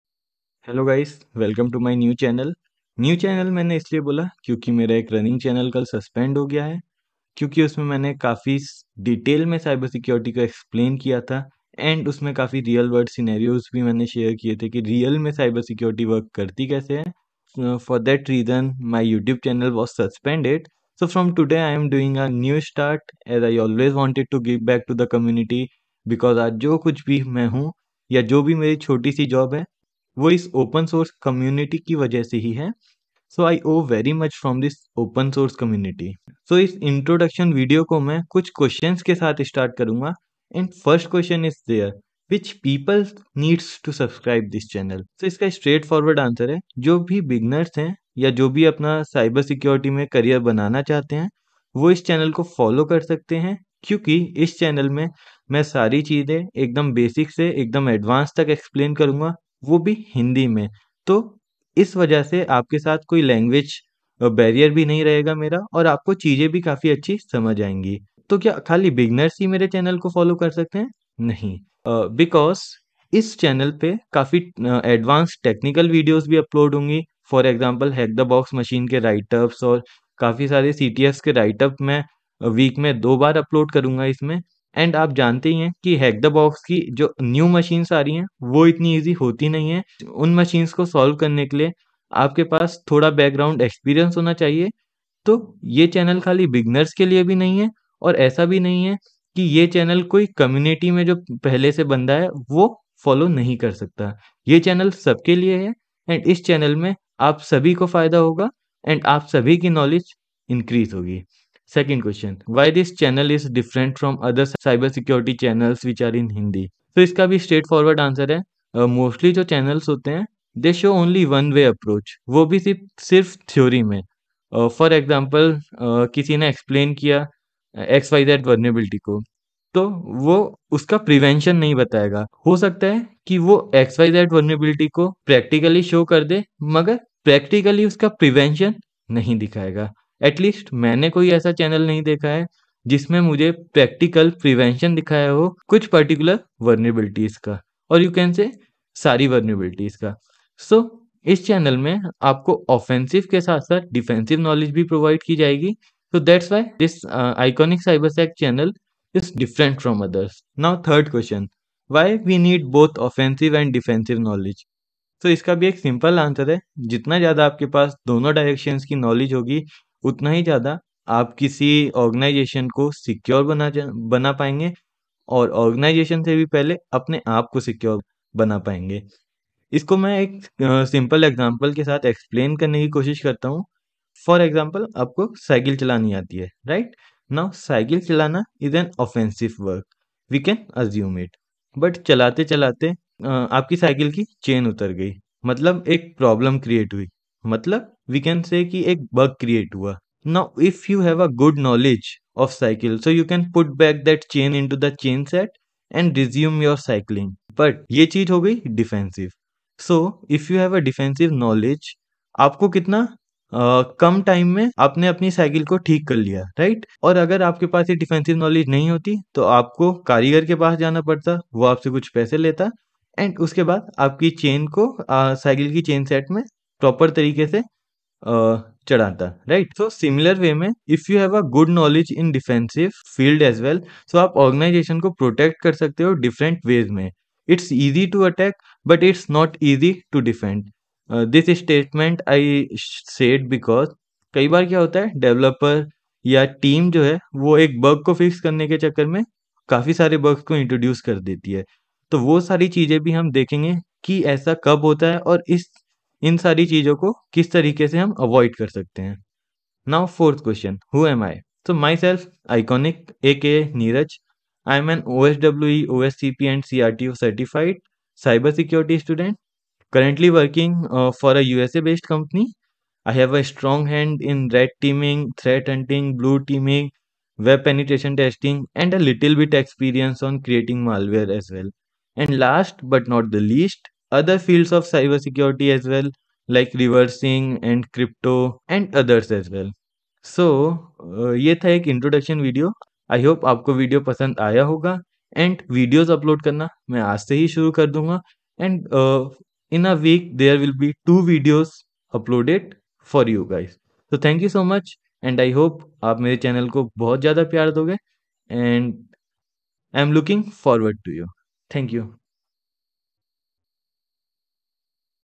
#3 [Hindi] Why this channel | Channel Introduction | Cyber-Security
My vision is to provide cybersecurity knowledge for free to the people of Republic of India. Because from my past experience I learnt that Institutes are fooling peoples to spend large money on their courses, in which they don't work on the thinking of the student they just focus to complete their courses.

That's why I starts this youtube channel. Hope you guys like it and feel free to reach me out on discord.